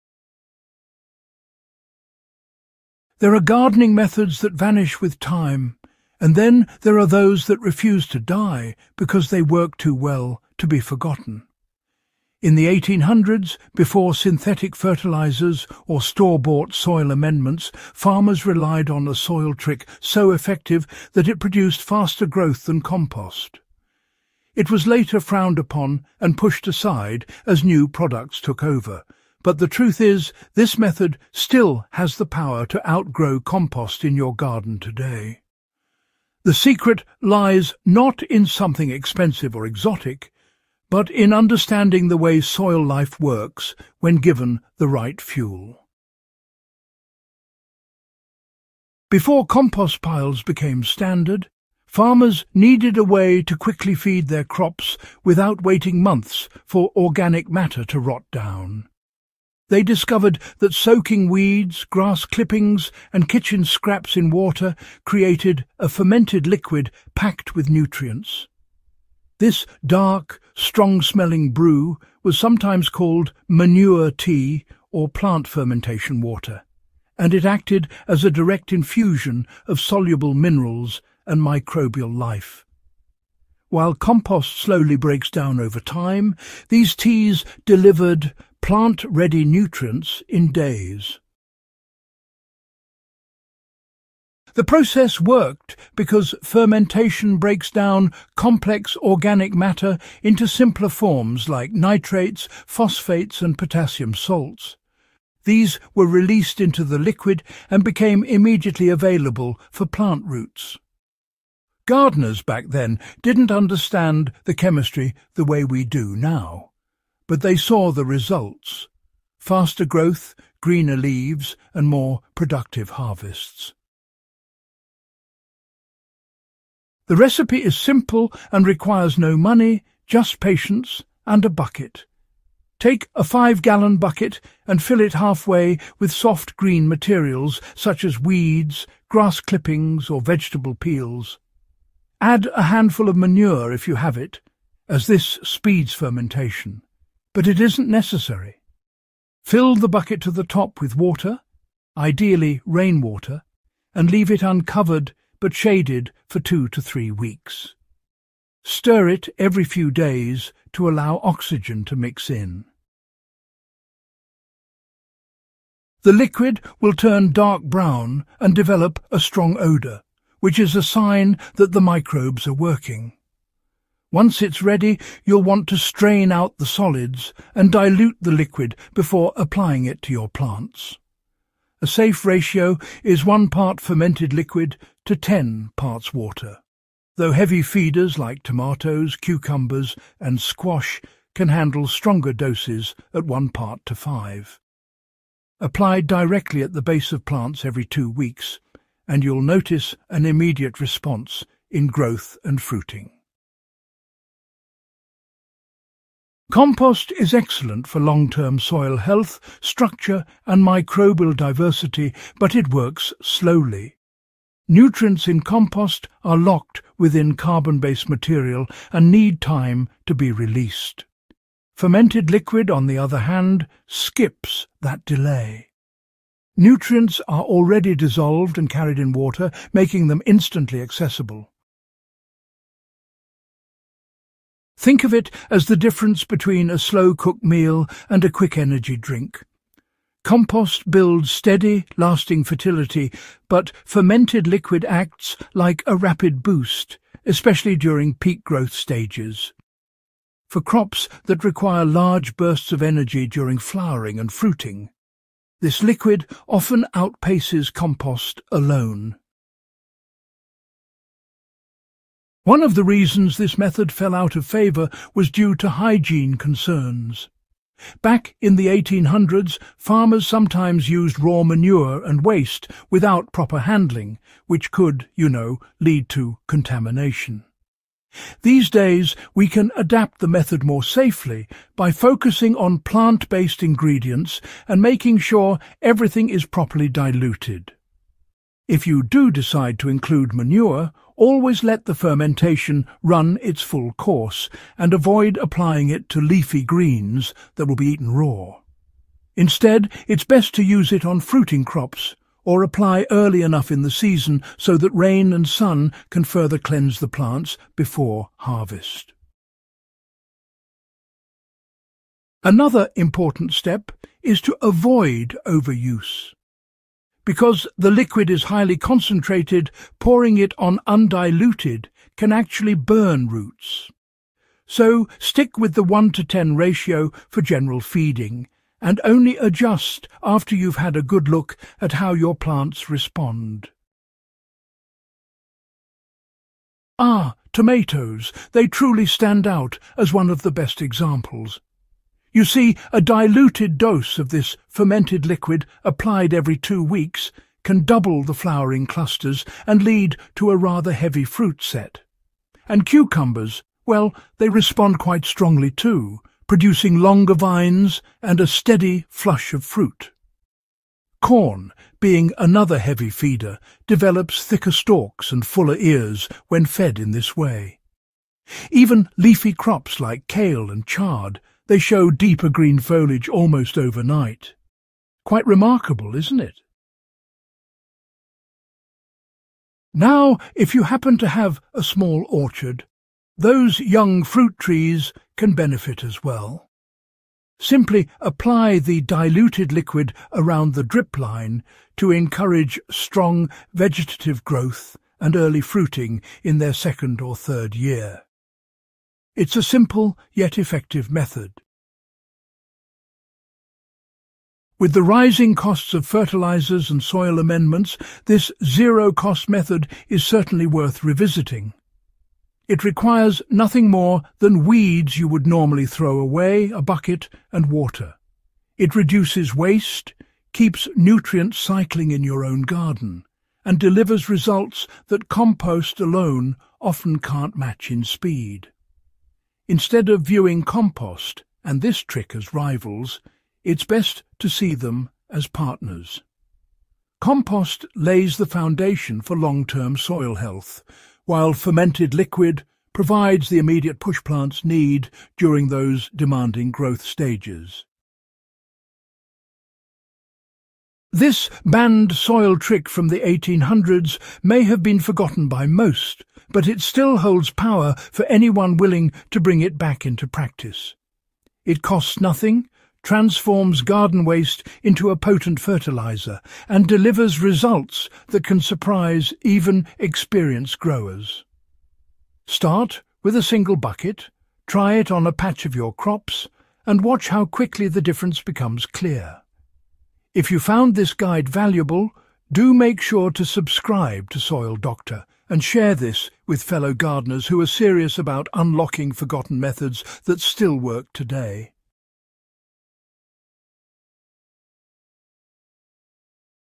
WHY THIS BANNED SOIL TRICK FROM THE 1800S STILL OUTGROWS COMPOST
In the 1800s, farmers used a soil trick so powerful it was eventually banned and forgotten. But when tested against modern compost, it still produces bigger, healthier crops faster. In this video, we uncover the controversial method and show why it still works today.

The strange historical soil hack that farmers swore by

Why it was banned and nearly erased from gardening lore

How it compares to compost in crop growth

The safe way to recreate it in your garden

📌 We Also Answer:

What old farming methods still work today?

Why was this soil trick banned?

Is it really more powerful than compost?

How can gardeners adapt it safely?

🌿 The Lazy Gardener’s Shortcut to Bigger Harvests
Sometimes the past hides secrets that modern gardening overlooks. This 1800s method proves nature always had the answers.